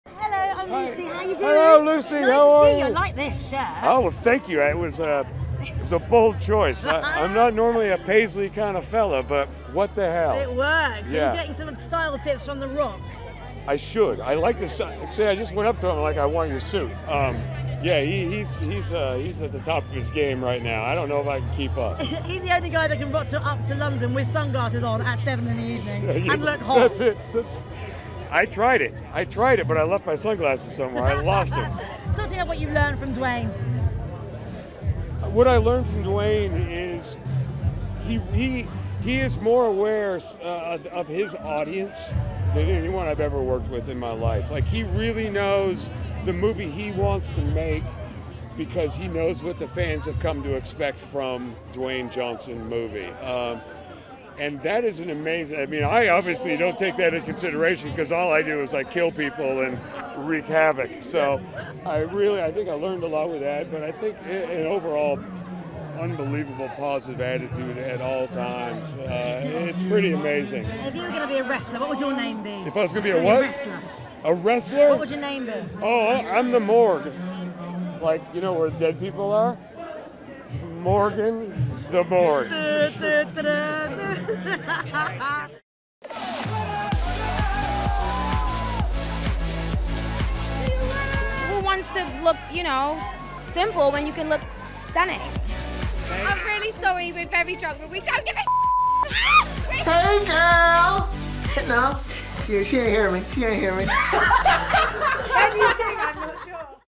Jeffrey Dean Morgan gushes over Dwayne Johnson
The incredible Jeffrey Dean Morgan reveals what his wrestler name would be while at the European premiere of Rampage. Report by Nikhita Chulani. Test